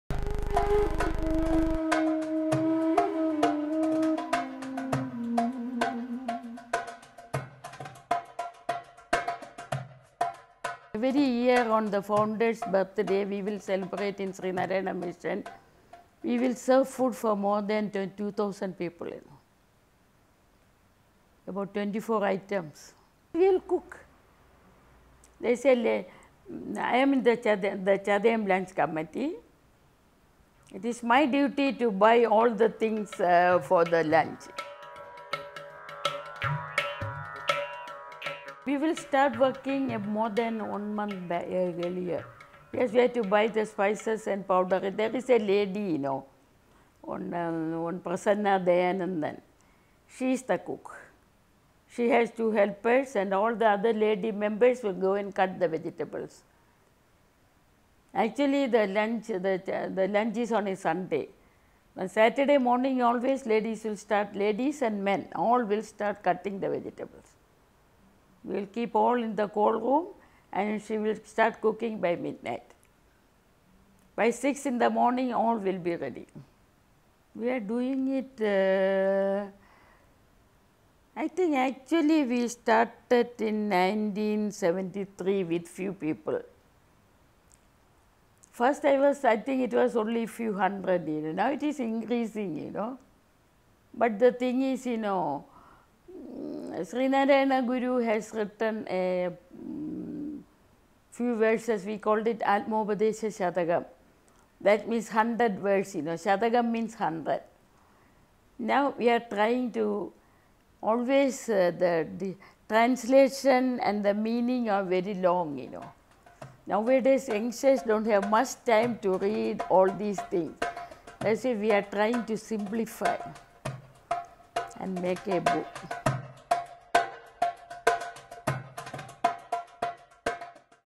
SUNANDA3/4 Serving Others
Mrs Devi has also been heavily involved with the annual Chathayam celebrations in Sree Narayana Mission. On the birth anniversary of Sree Narayana Guru, a traditional Chathayam lunch is organised comprising of 24 traditional vegetarian dishes. Starting in 1973, Mrs Devi has seen the crowd grow from several hundreds to a couple of thousand. The event is in fact so popular that they had even run out of food last year! The menu is decided solely by the volunteers and everything from the ordering of the necessary ingredients to the preparation and cooking of the food is done by them. Ordering the needed to prepare meals for thousand over people is no easy feat, but Mdm Sunanda has been successfully doing so and takes pride in all that she has accomplished.

As part of her other duties in the other subcommittees, Mdm Sunanda has been involved in efforts to simplify and streamline spiritual material to make it more accessible to youth in light of falling participation by the younger generation in spiritual discussions. As a result of the various committees she’s involved in, Mdm Sunanda mentions that where she once used to visit residents and spend time with them, she finds that unfortunately she has less time for such activities. Stressing the importance of just interacting with the residents, she emphasizes that no extravagant gestures are needed and often simply spending time with them is sufficient to make a difference. She in turn is also touched as these residents remember her and often ask after her should she happen to miss a session.